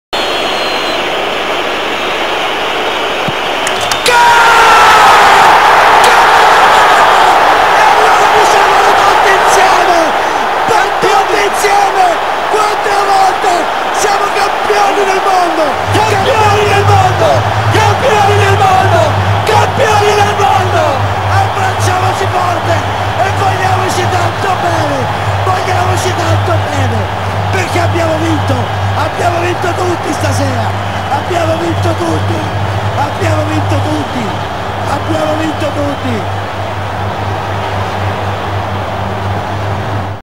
Fabio Grosso scores the last penalty in 2006 World Cup Final
Fabio Grosso scores the last penalty against France. Italy wins 5-3 and captures the four star, the four WORLD CUP! Comment by Fabio Caressa and Giuseppe Bergomi of Sky Sport Italia.